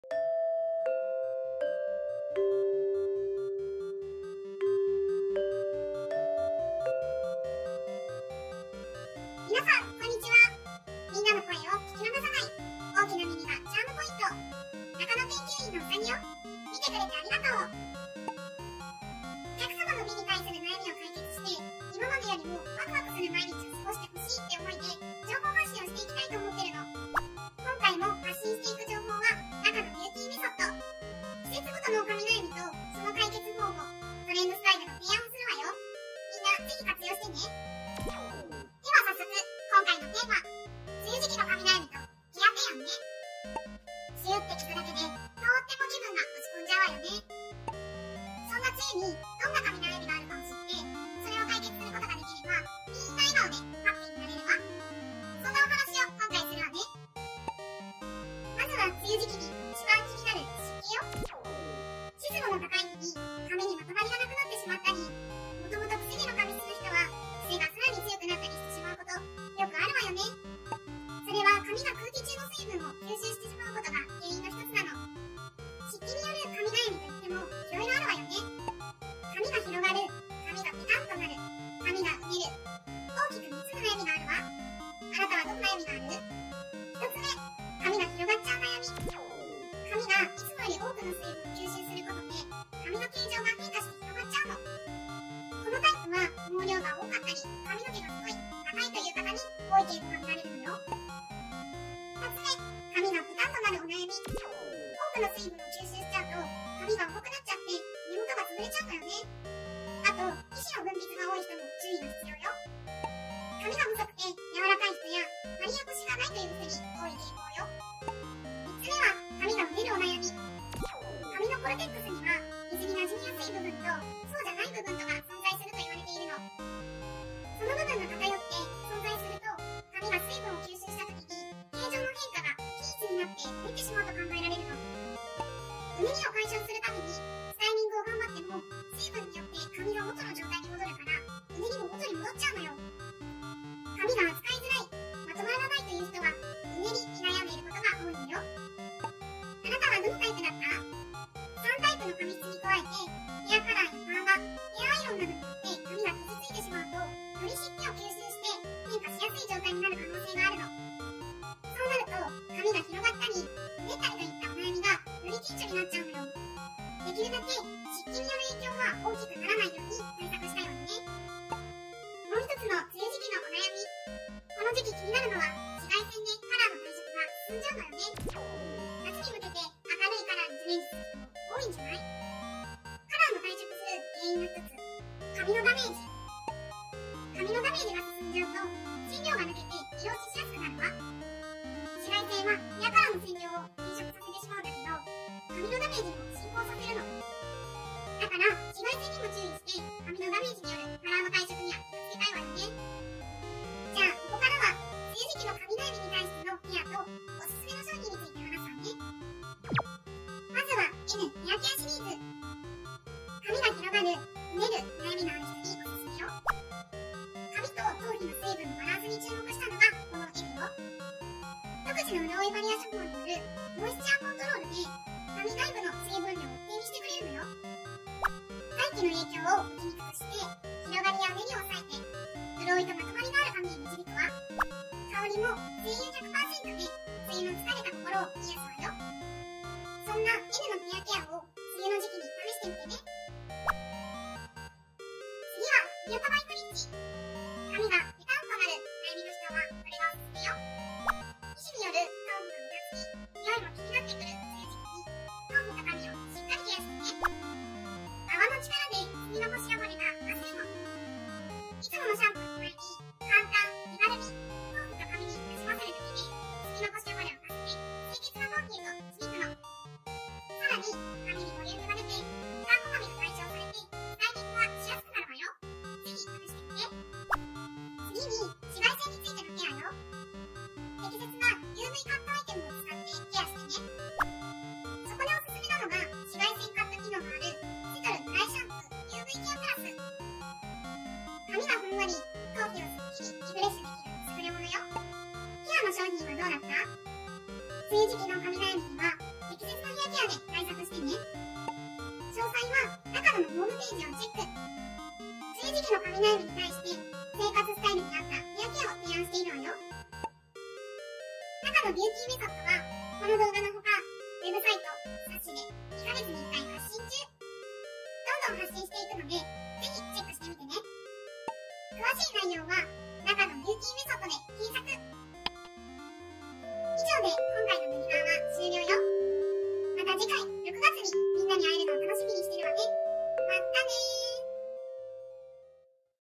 髪悩みまるっと解決ウサちゃんねる ～あなたの髪悩みを解決するウサギ研究室～
【ISSUE8】
今回のテーマ
「梅雨時期の髪悩みとケア提案」
髪悩みと言っても様々な悩みがあって、それぞれケアの方法が・・・
一人一人に合ったケアの方法やケアアイテムを紹介。

0:00　オープニング
0:39　テーマ・コンテンツ紹介
0:58　梅雨時期の髪悩み
3:24　ケアにオススメの商品
5:12　NAKANO BEAUTY METHODについて

■関連サイト